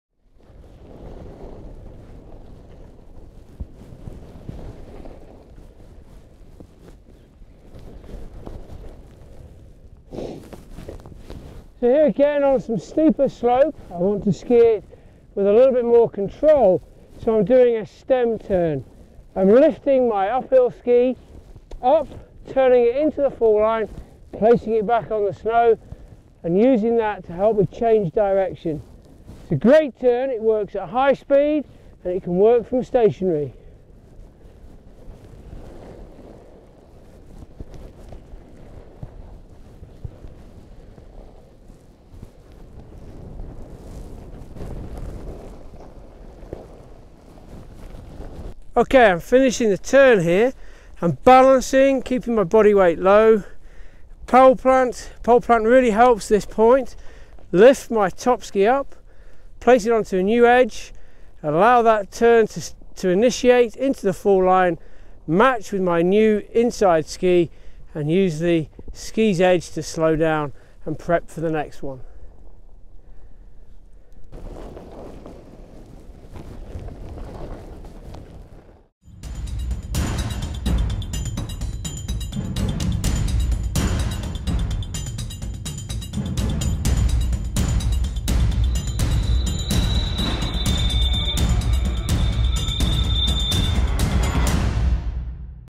Ski Touring - Stem turns
Glenmore Lodge instructor Andy Townsend demonstrates the stem turn, excellent for dealing with steeper terrain.  Glenmore Lodge offer a range of avalanche and ski touring courses from January to April, based in the heart of the Cairngorms National Park, Aviemore as well as several ski touring trips to the Alps.